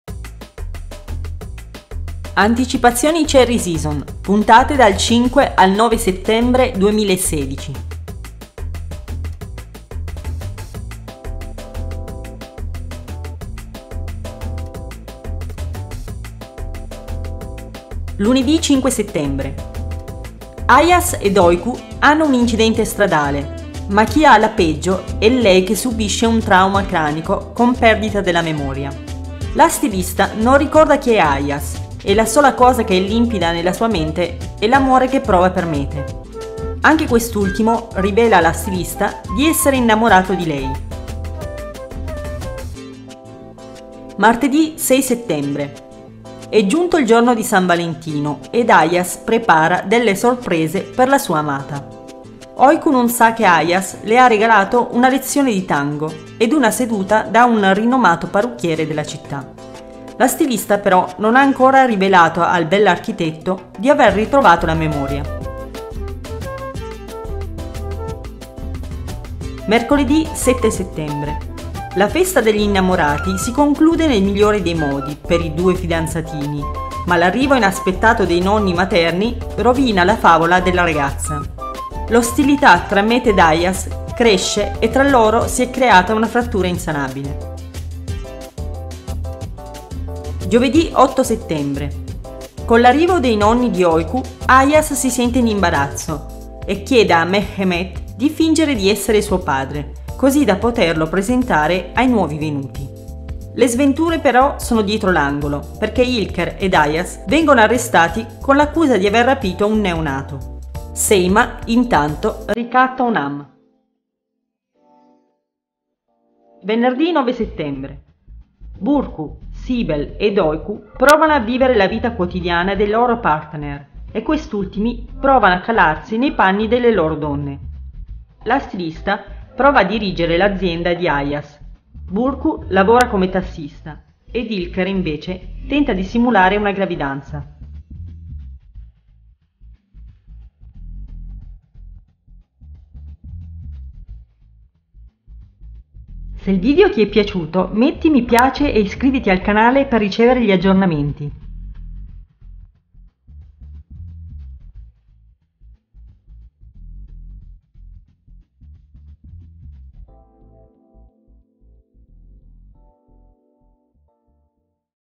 Cherry Season in Italiano: Anticipazioni puntate dal 5 al 9 Settembre 2016
Anticipazioni Cherry Season. Trama puntate Cherry Season in Italiano da lunedì 5 a Venerdì 9 Settembre 2016.